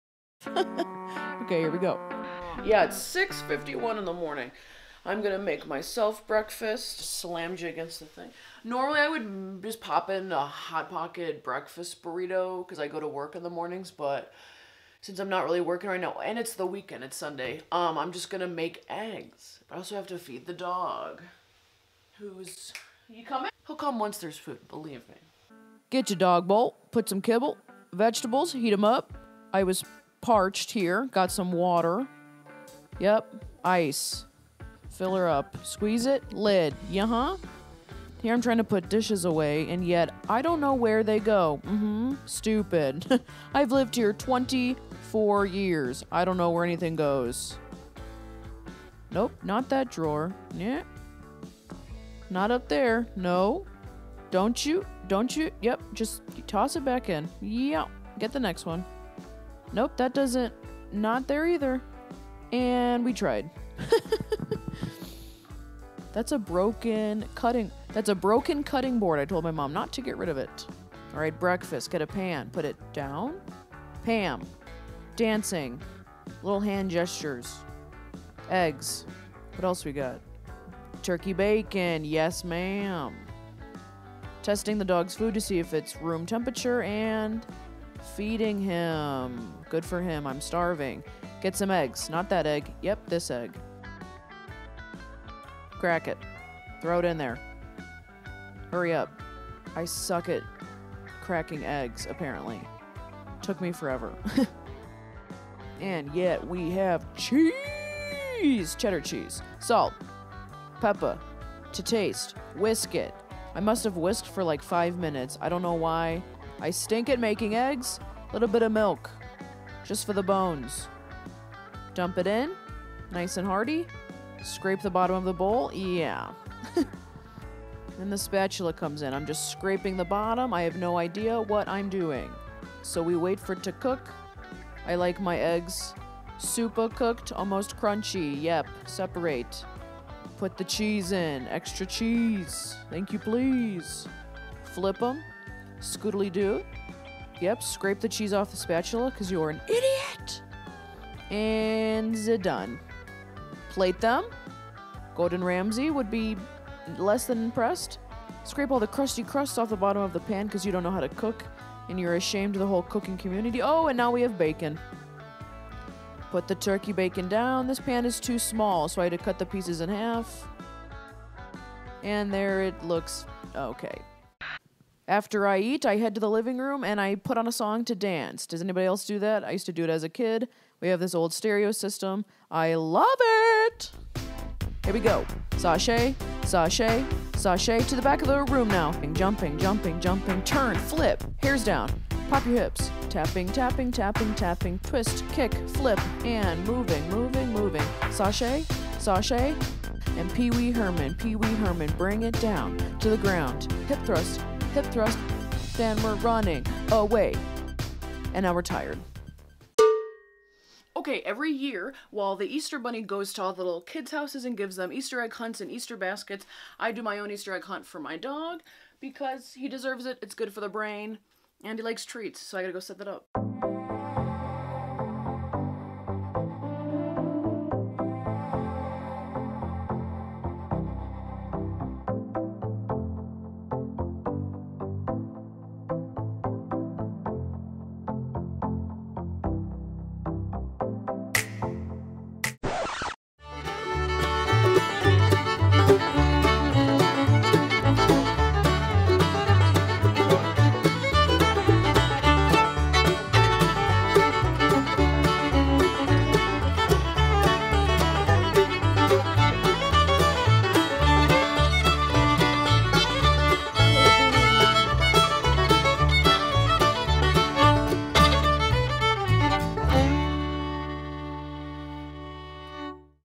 Tupperware Dilemma and Almost Crunchy Eggs - Vlog #1
Content includes behind-the-scenes clips, bloopers, extra footage, community chats, monthly niche video uploads, and MORE!! This is the first, of hopefully many, VLOGS !

These aren't meant to be necessarily funny - these are just snippets of my life ! Enjoy!

Cameo: Christines_Snaps